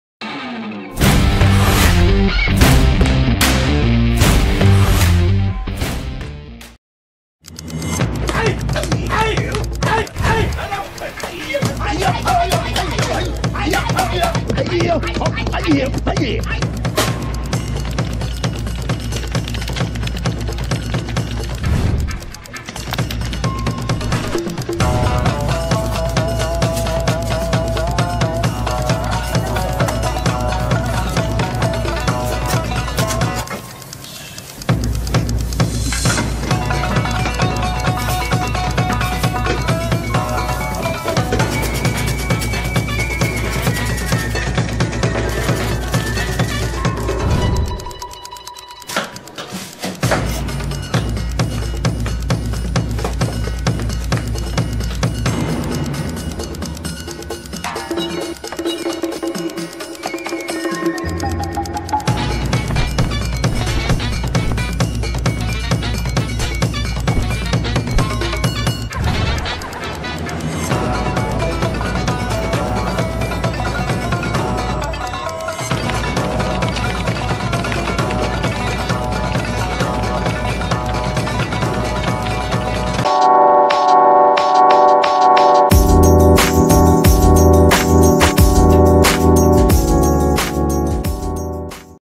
Amazing people at work they have skills nr3
Some staff is just every boss dream to have in the job. Like this Indien people att the shirt pick up store.